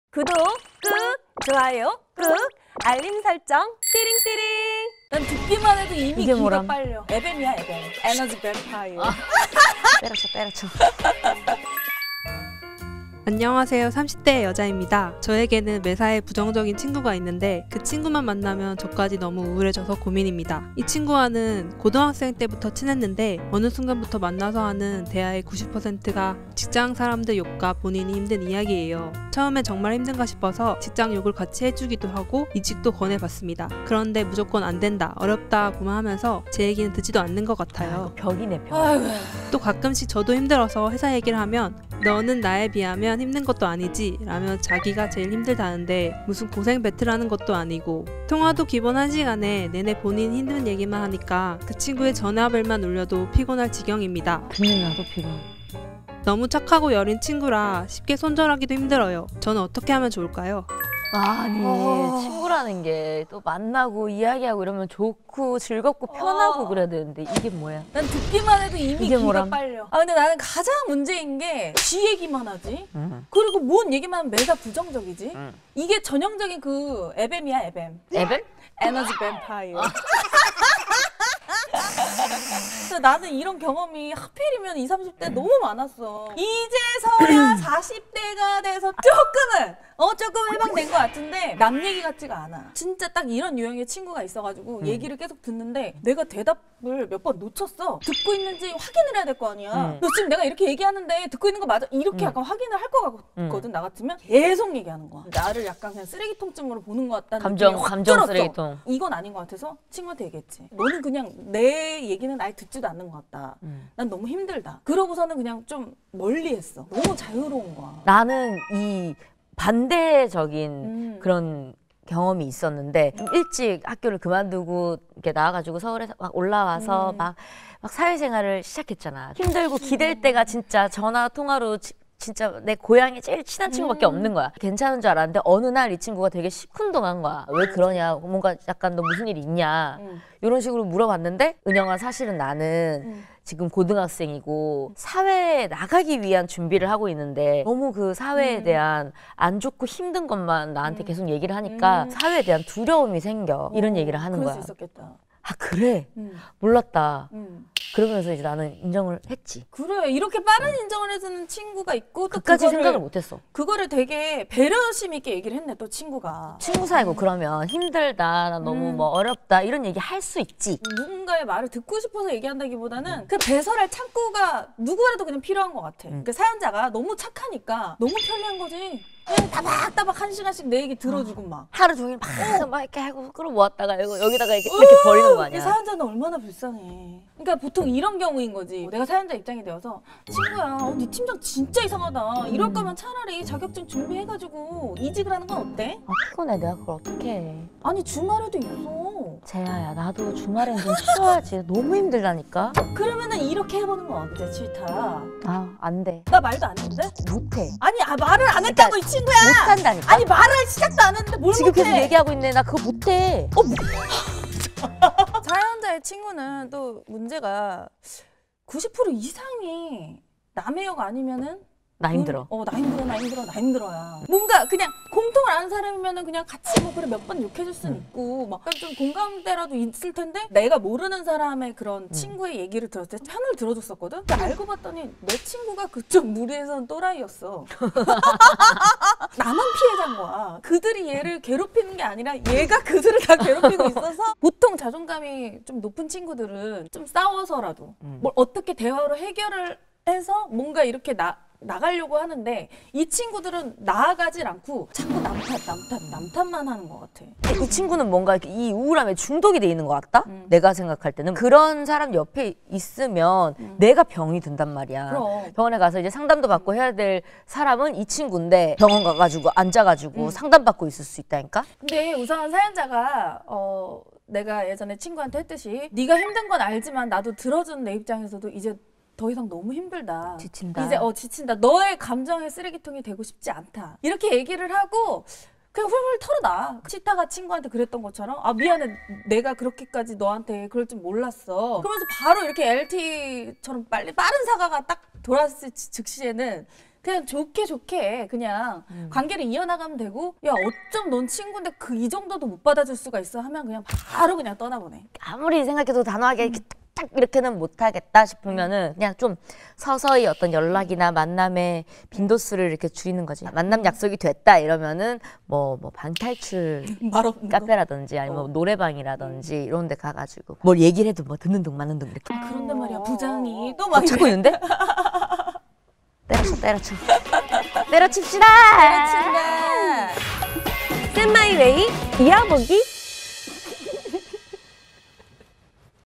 나를 감정 쓰레기통 취급하는 친구, 손절만이 답인가요? | 에너지 뱀파이어 대처법 ep.95 《쎈마이웨이》
자기 힘든 얘기만 주구장창 늘어놓는
에너지 뱀파이어 친구 때문에 미치겠어요

제 말은 귓등으로도 안듣지만
착하고 여린 친구라서 손절은 못하겠어요. 어떡하죠?